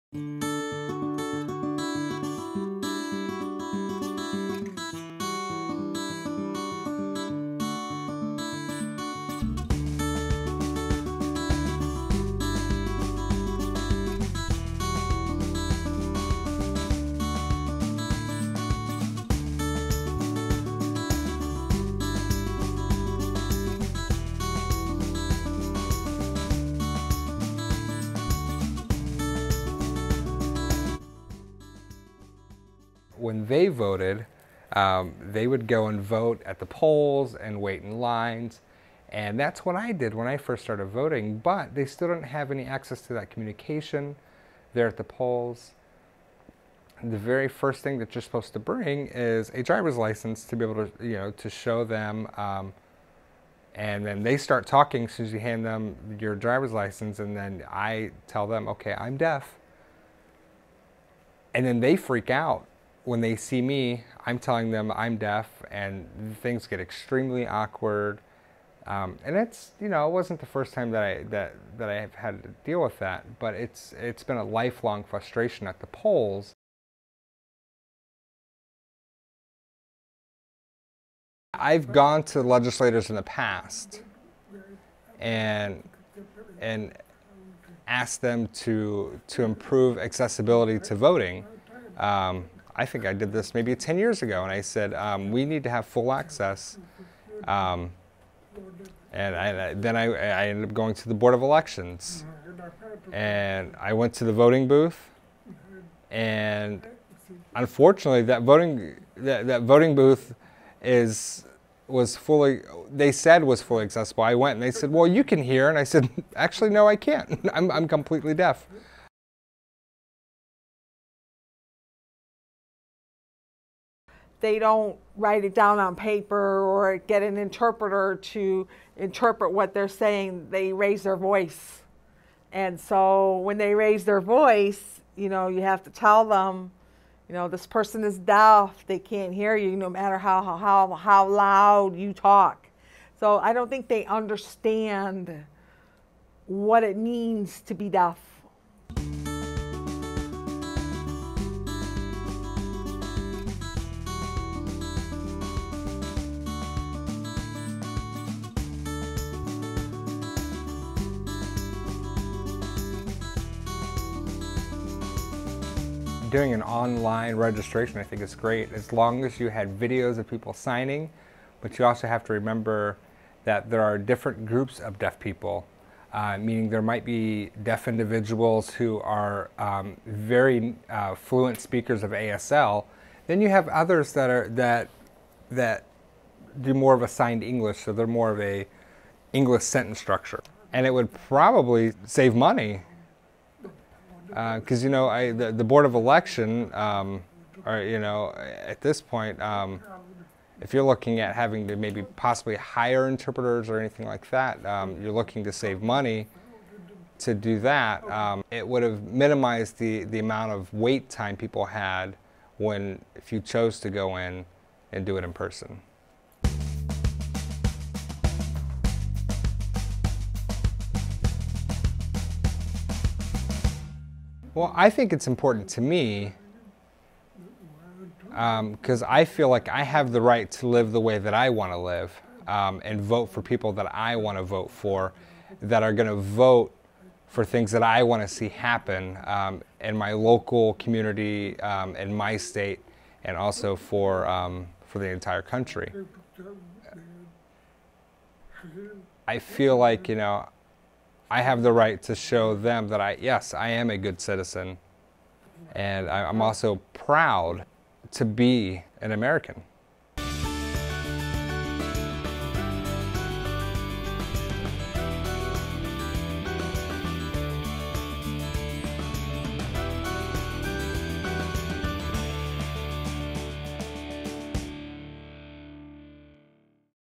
Online for All: Bridging the Divide for Deaf and Deaf-Blind Voters
Deaf and deaf-blind voters are often overlooked by officials and constantly encounter problems while trying to cast their ballot. Online voter registration is one way to make the system easier for these voters—but the websites must be accessible to them.